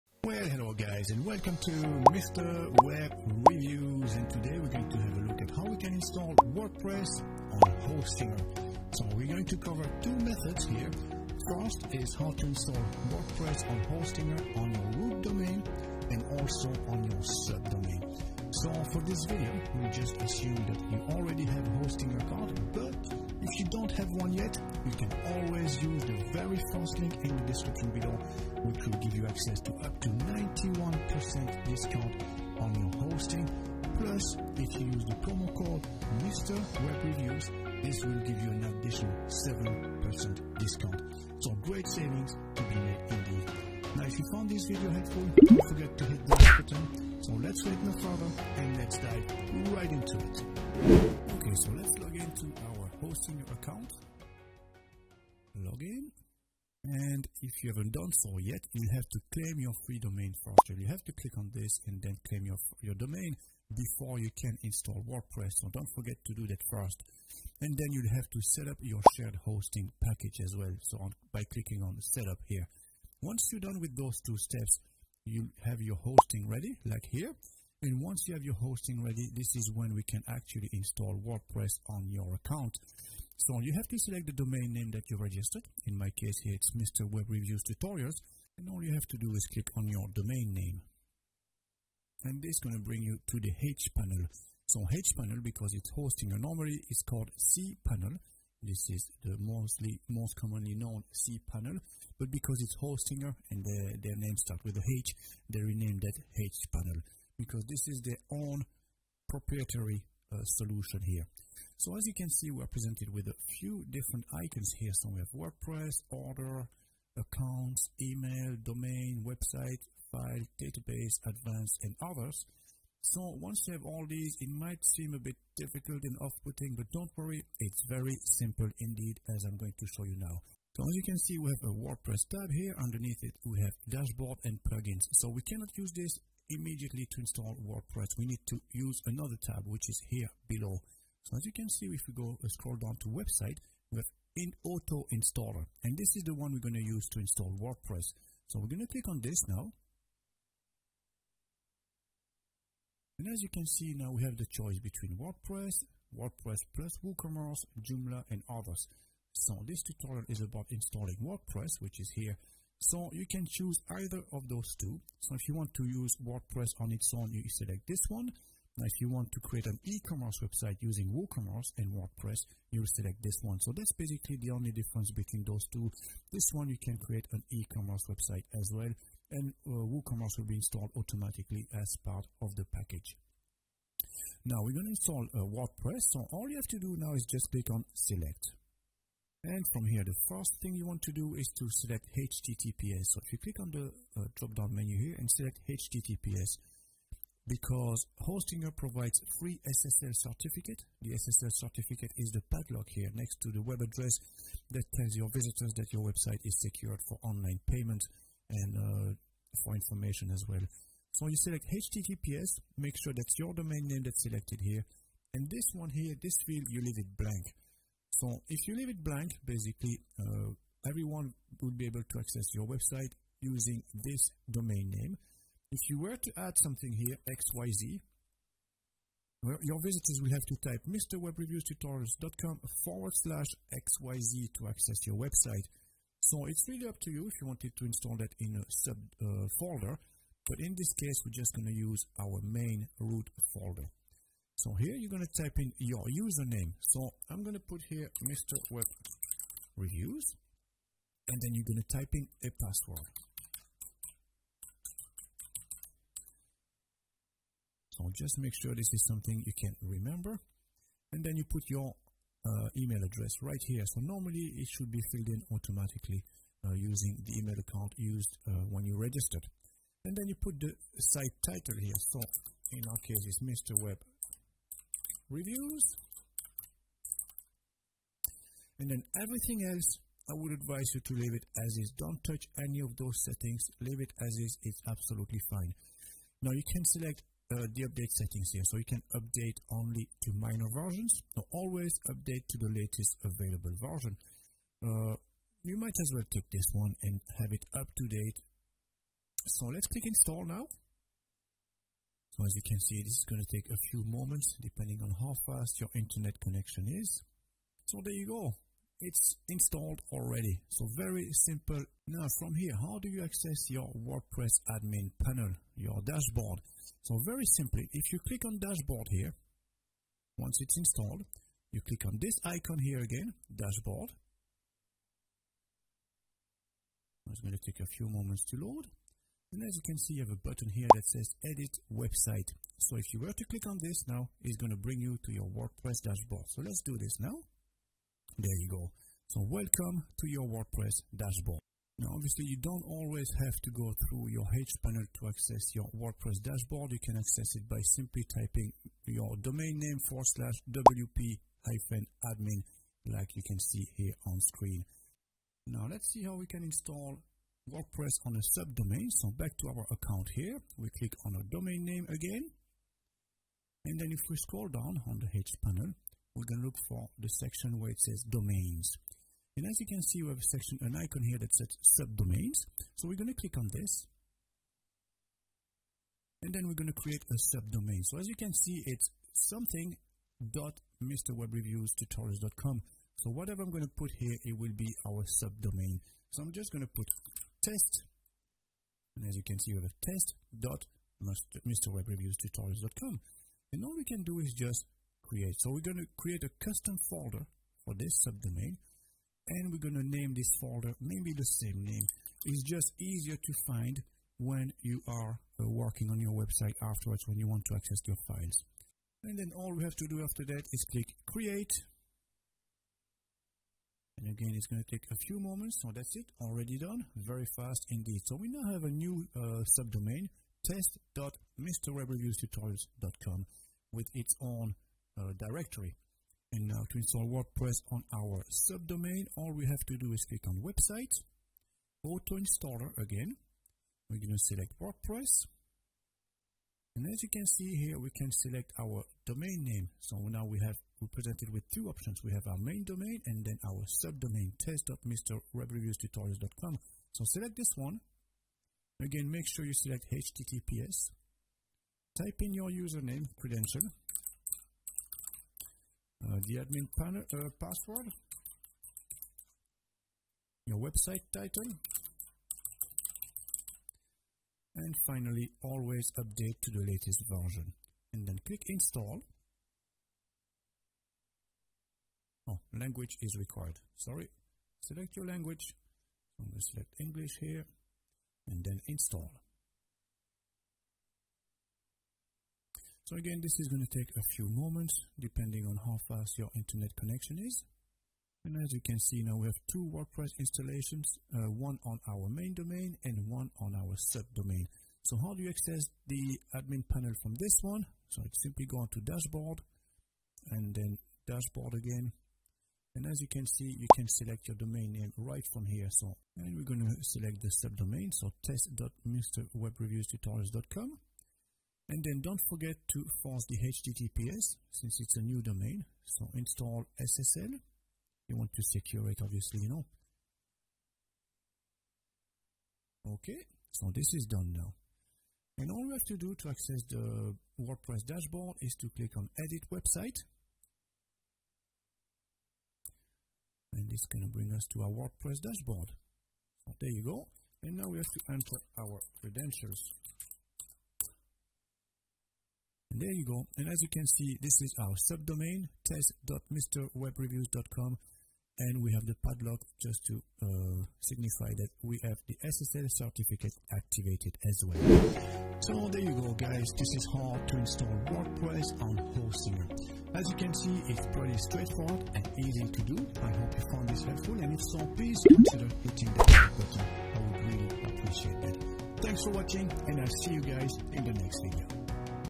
How to install wordpress on hostinger and on hostinger subdomain
Hey Guys! Today In This Video we are going to look at how to install Wordpress on Hostinger but also at how to install Wordpress on a Hostinger SubDomain.

▶ TIME STAMPS FOR:
0:00 Intro to how to install Wordpress on Hostinger
0:57 How to install Wordpress from the Hostinger HPanel
5:55 How to Install Wordpress on Hostinger SubDomain
9:14 Conclusion